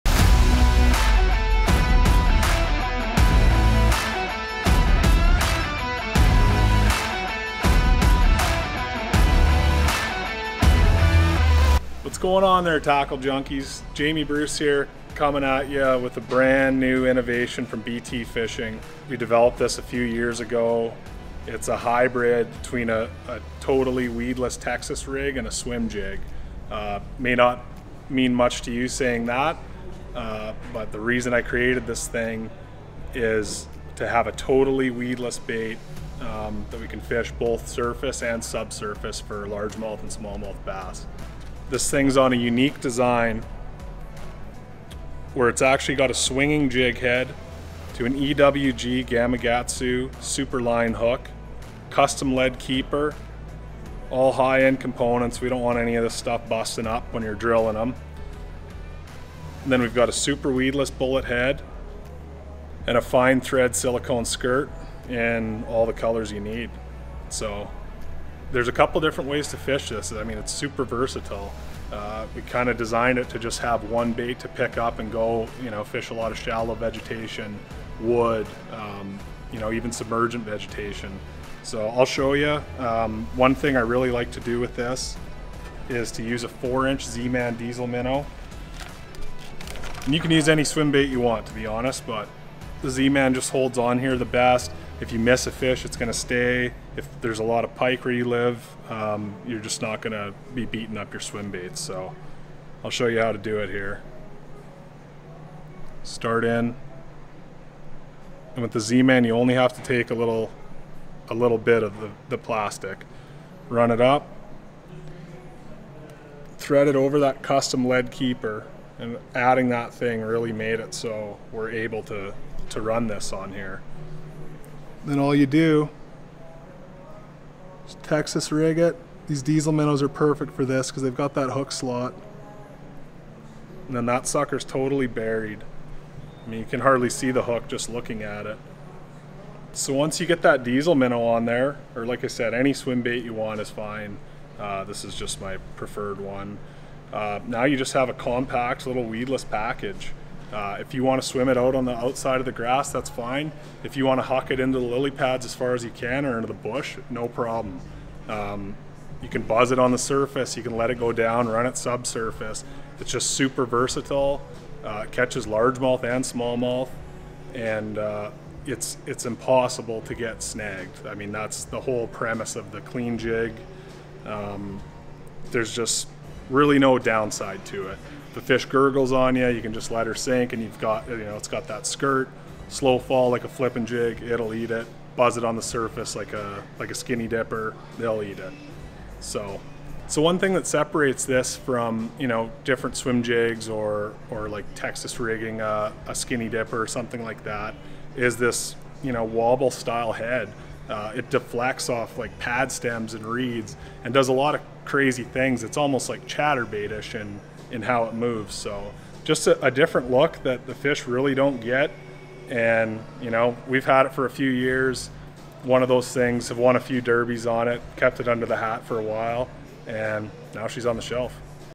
Jamie Bruce's NEW Clean Swim Jig!
Here's a quick video of Jamie releasing his new to the market clean swim jig! Built with quality components such as Gamakatsku EWG hooks, thin threaded silicone skirts and custom lead bait keeper, this jig will help you put more smallmouth & largemouth bass in the boat this open water season.